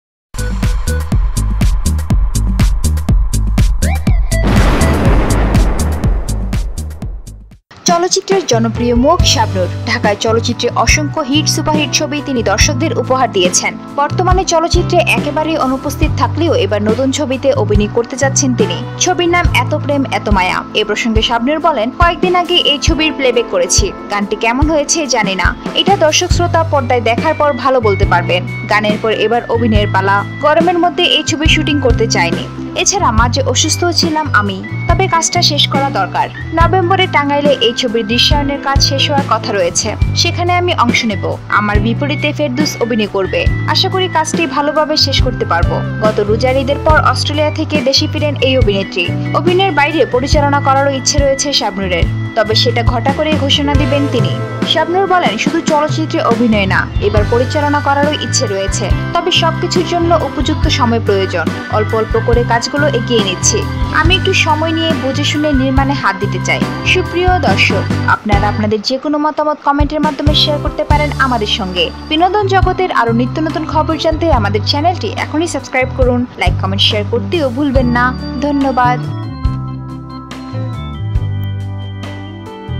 ফেরদৌসকে সঙ্গে নিয়ে কাজে ফিরছেন শাবনূর !!!- Shabnur Is Back
চলচ্চিত্রের জনপ্রিয় মুখ শাবনূর। ঢাকাই চলচ্চিত্রে অসংখ্য হিট-সুপারহিট ছবি তিনি দর্শকদের উপহার দিয়েছেন। বর্তমানে চলচ্চিত্রে একেবারেই অনুপস্থিত থাকলেও এবার নতুন ছবিতে অভিনয় করতে যাচ্ছেন তিনি। মোস্তাফিজুর রহমান মানিক পরিচালিত এ ছবির নাম ‘এত প্রেম এত মায়া’।